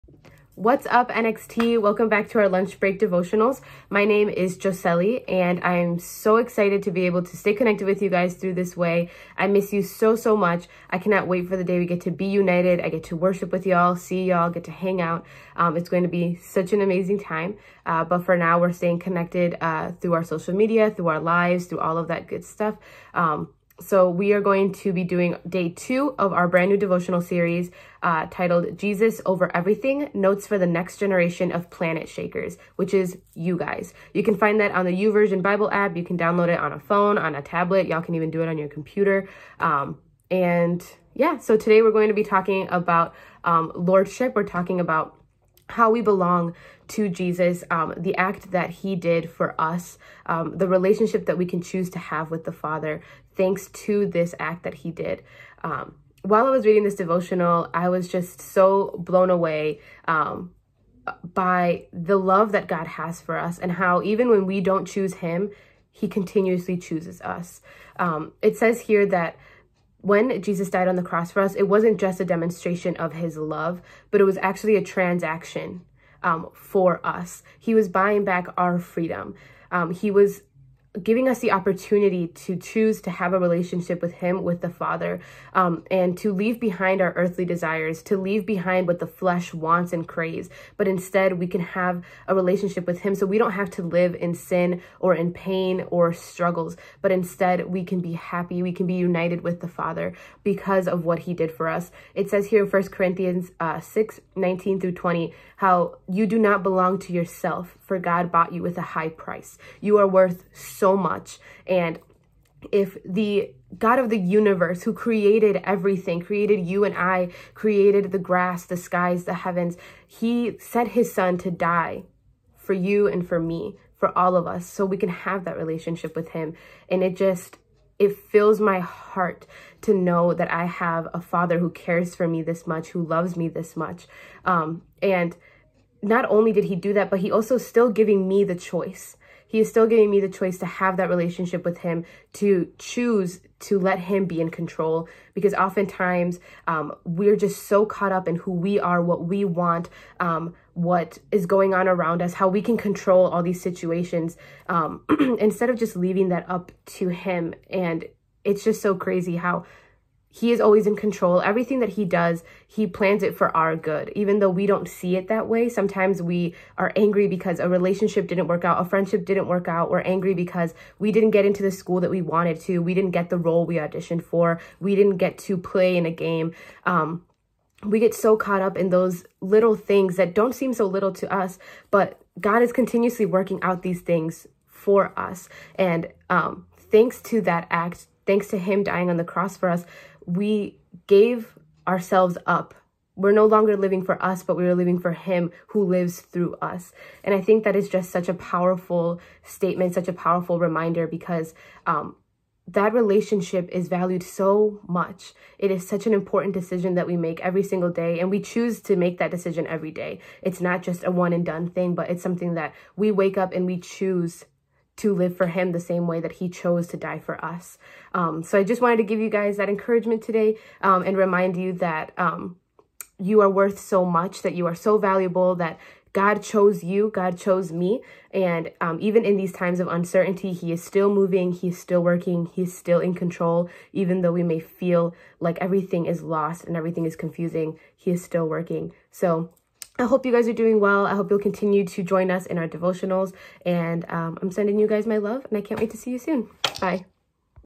Lunch Break - Jesus Over Everything, Day 2 - April 23, 2020
NXT Sugar Grove Life Group Leader, Josely Hernandez shares from the devotional, Jesus Over Everything, Day 2!

Download the YouVersion Bible App, and search for "Jesus Over Everything" in the Plans section to follow along!